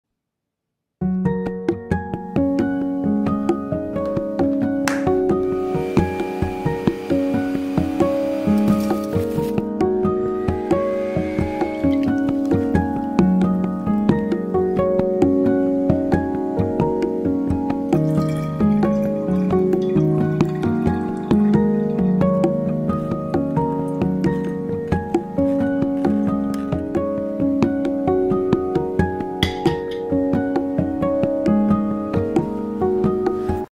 How to make verjuice ?! Best salad dressing
Verjuice is basically the juice of unripe grapes.

The color of the homemade verjuice might change from green or yellow to brownish. It is okay, the taste will still be good.

You can use it in any dish that is calling for lemon juice, lime juice or vinegar. I often use it as a dressing in our favorite Persian salad called Shirazi salad. You can use it in marinades and cooking too. I like adding some in soups. It has the same effect as a splash of lemon juice.